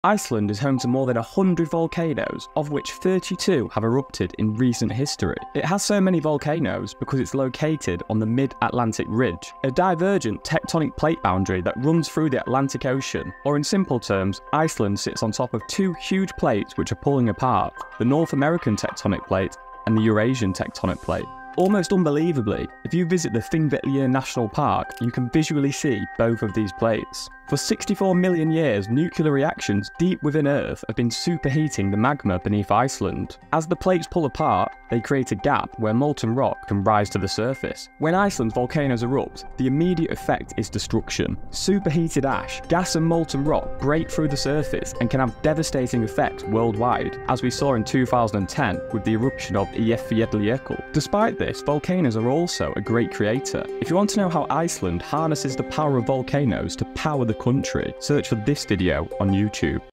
Why does Iceland have SO MANY Volcanoes?
Iceland is VERY Volcanically active, as is currently in the news, but this is why!